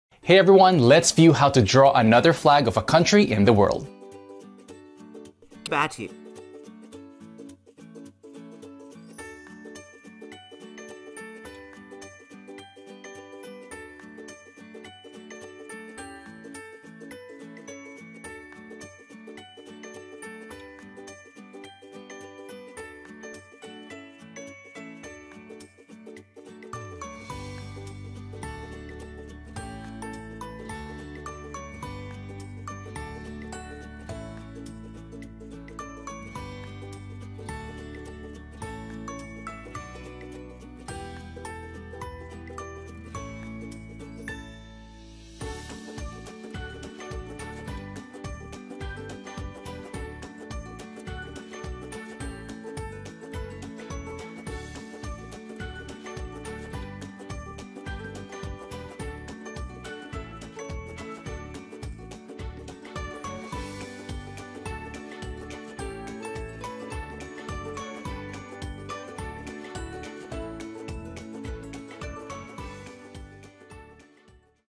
Flag of Kiribati - 🌍 How to Draw the Kiribati Flag - SVG Vector
- Flag of Kiribati: Check out how to draw the flag of Kiribati. Let’s discover how to draw the national flag of Kiribati in a few minutes. Discover the national flags of all the countries in the world.